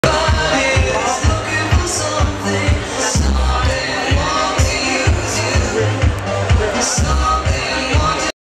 La musique était sensationnelle au party du Gala des Artis 2010
J'ai rencontré la belle "Lady diskjokey" du Party